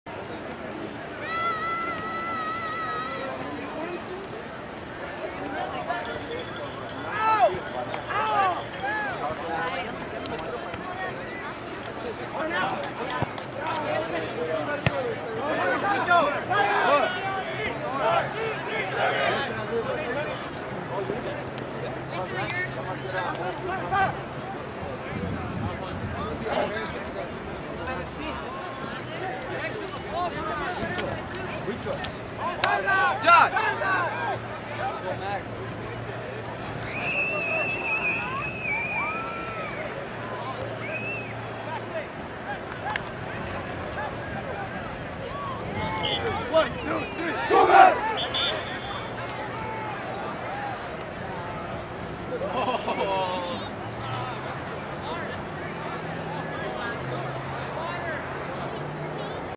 20121201 HS Varsity Boys Liberty v McFarland cheer
Liberty High Bakersfield Soccer Varsity Tournament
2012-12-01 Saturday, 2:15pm
Liberty Park South

BrownCowSoccer.com -- Local soccer videos, pictures, news, and more.